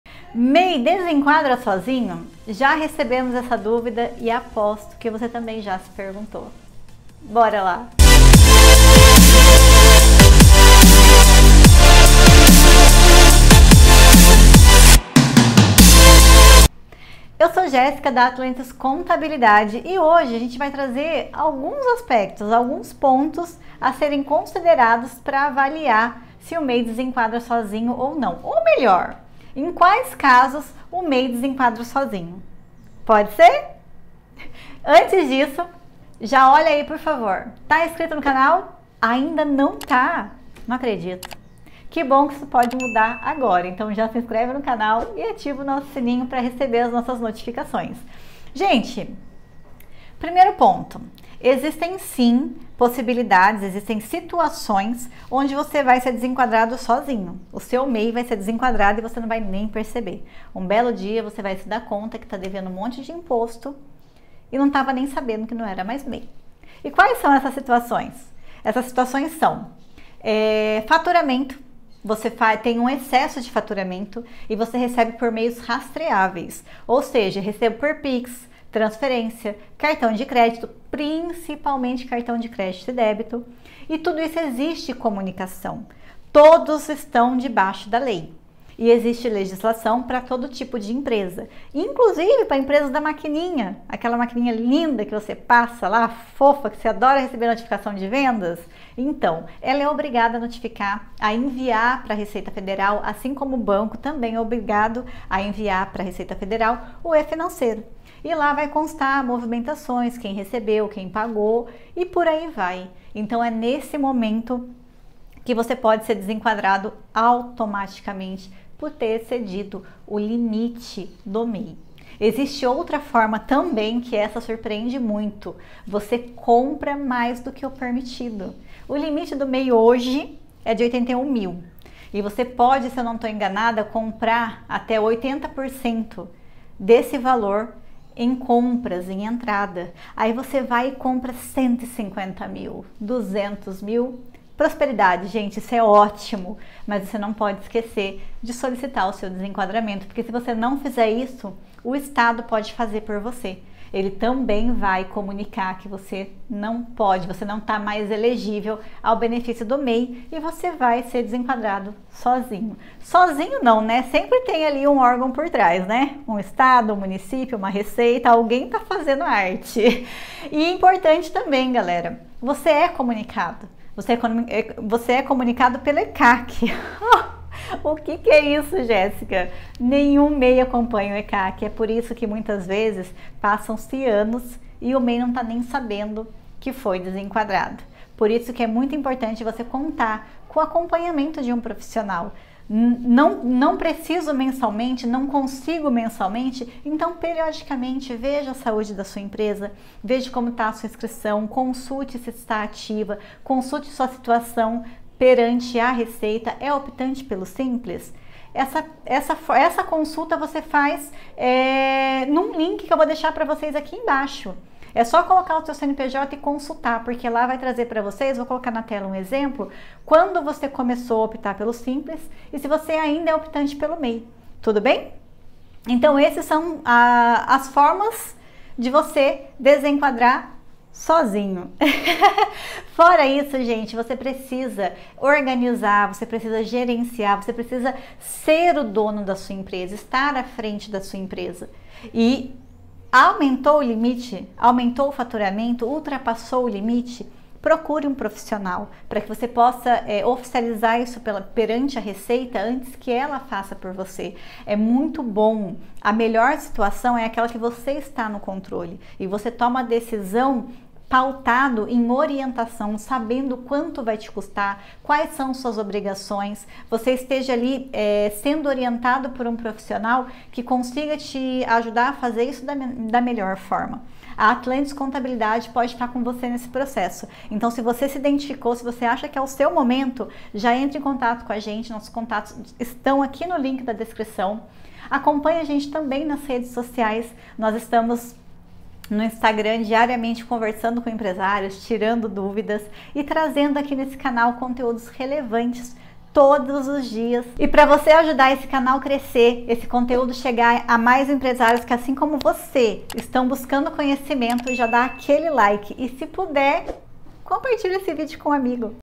MEI DESENQUADRA SOZINHO?
Você tem um MEI mas tem dúvidas se ele desenquadrou ''sozinho''? Nesse vídeo explicamos por quais motivos o seu MEI pode ser desenquadrado sozinho e como você pode evitar isso.

Como prometido no vídeo, esse é o link para consultar se o seu CNPJ ainda consta como MEI

Aproveita para seguir a gente no Instagram: @atlantiscontabilidade e @jessyca.talita garanto que não vai se arrepender!